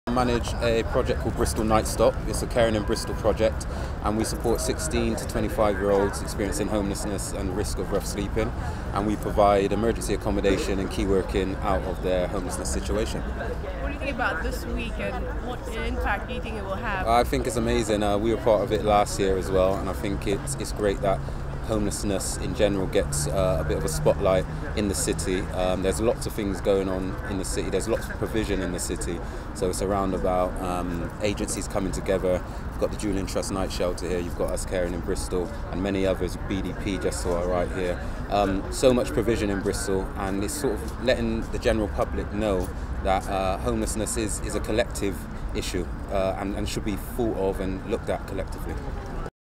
Tyrell Jarret Bristol Night Stop
UWE News interviewing one of the partners on homelessness in Bristol about the importance of the activities at the Bear Pit